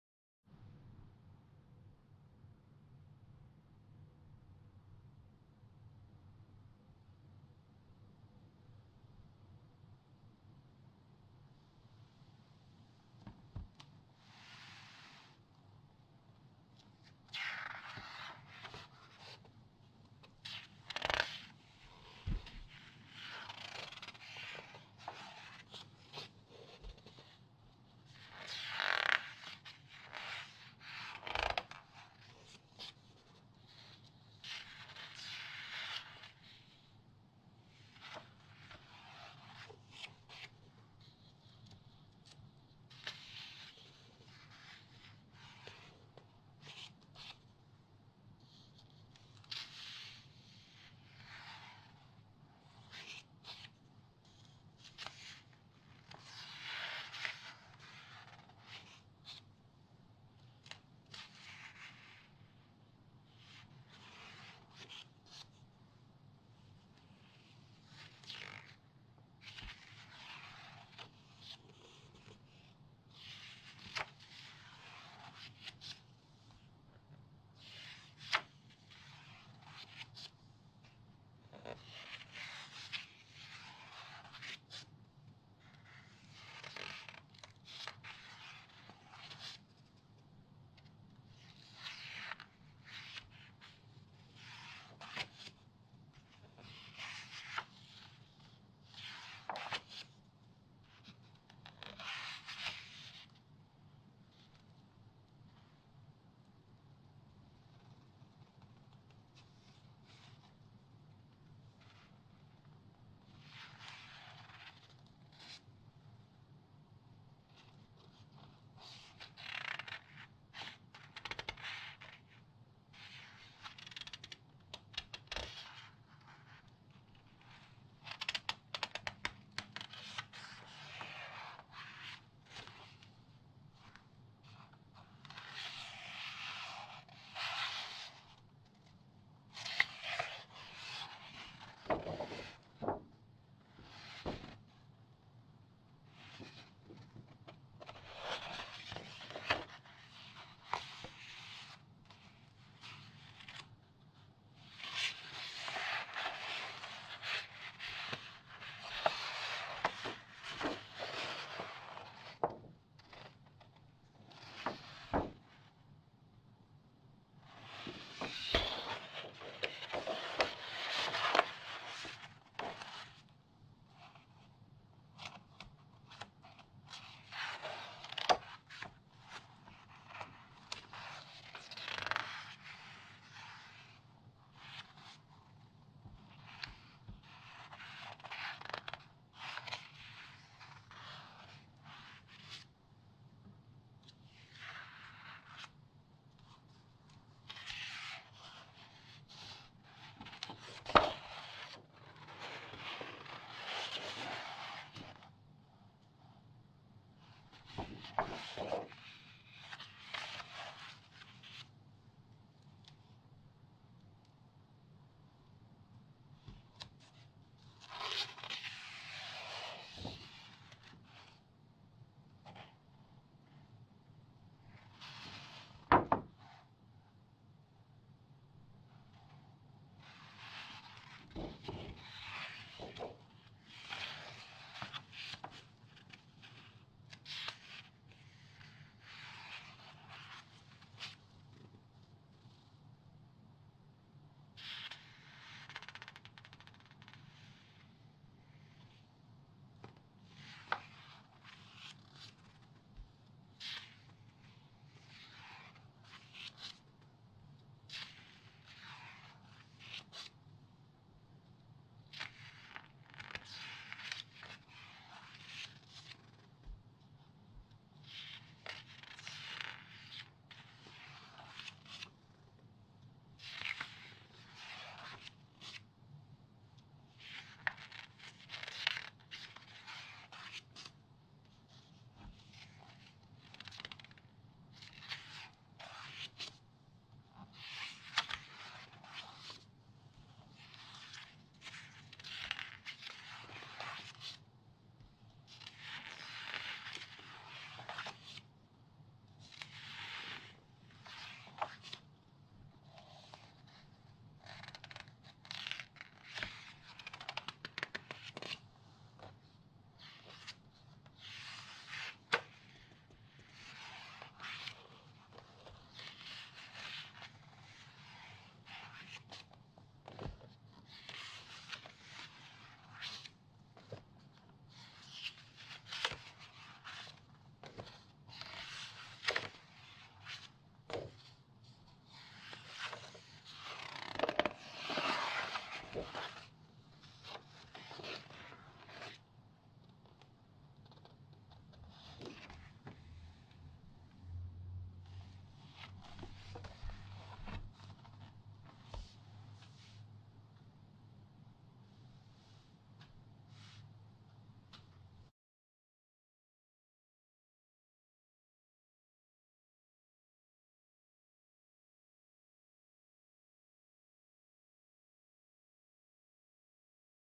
1962 September ASMR Magazine Flip Through: Novita Cardin, Lanvin, Griffe, Dior, Ricci
Vintage issue of Italian Novita September 1962.  Relaxing ASMR sounds from page turning and a step back in time at the same time.

Fall fashions from Nina Ricci, Cardin, Dior, Simonetta, Lanvin Castillo, Griffe, Heim, Saint Laurent.  Se Ne Parla.  La Bella Ritorna in Citta. Nostalgia del Ricciolo. L Modelli dei grandi sarti per voi.

Ads: Orlane, Maidenform undergarments, Cosserat, Zucchi, Christian Dior fur, Delfion, Lyle and Scott, Boulestin cognac, Triumph undergarment, Saint Joseph, Glma fur

It would help the channel out.  Thank you for your support!